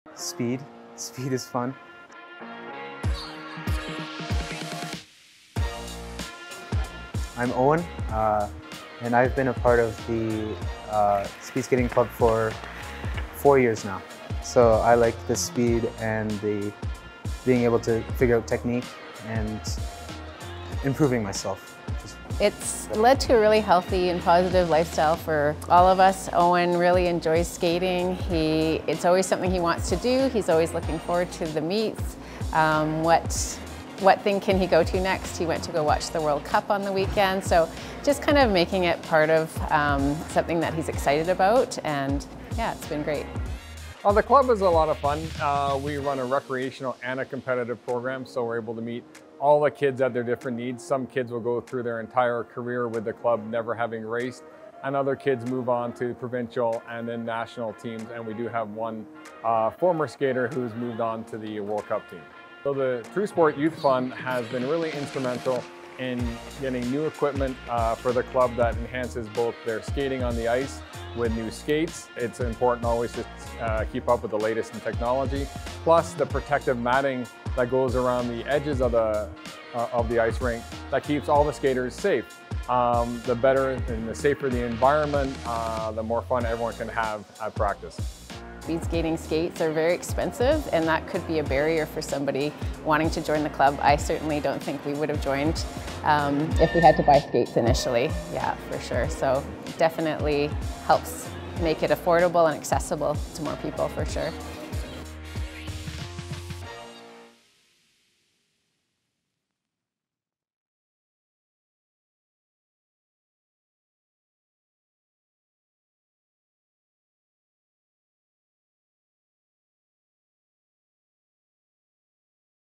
True Sport Youth Fund - Banff Canmore Speed Skating Club
What does it take to go fast? The right training and equipment, and a safe and welcoming environment to practice, says Banff Canmore Speed Skating Club Coach, Ian Mellors.

The Banff Canmore Speed Skating Club offers recreational and competitive programming focused on helping kids and adults build their skills safely and effectively. Thanks to the True Sport Youth Fund, they were able to provide athletes with the modern equipment and protective mats needed to get into gear.

The True Sport Youth Fund helps sport and recreation organizations based in the Bow Valley with annual financial assistance.